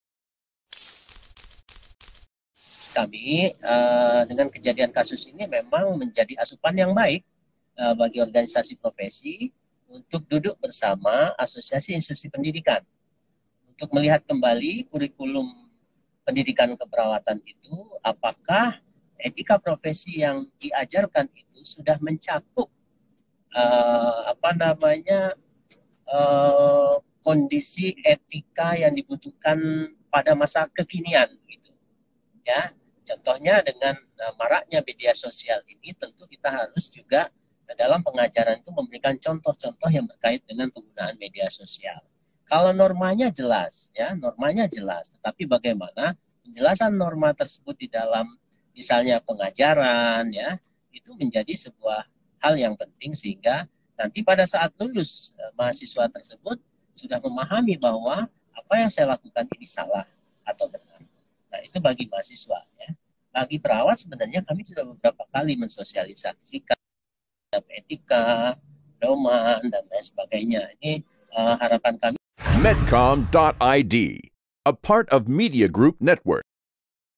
PPNI akan Evaluasi Kurikulum Pendidikan Keperawatan
Ketua Umum Persatuan Perawat Nasional Indonesia (PPNI), Harif Fadillah mengatakan bahwa dengan beredarnya sebuah video yang menunjukkan seorang perempuan bermasker tengah memasang kateter urin pasien laki-laki, Pihak PPNI serta berbagai organisasi maupun asosiasi pendidikan akan mengevaluasi kurikulum pendidikan keperawatan di Indonesia.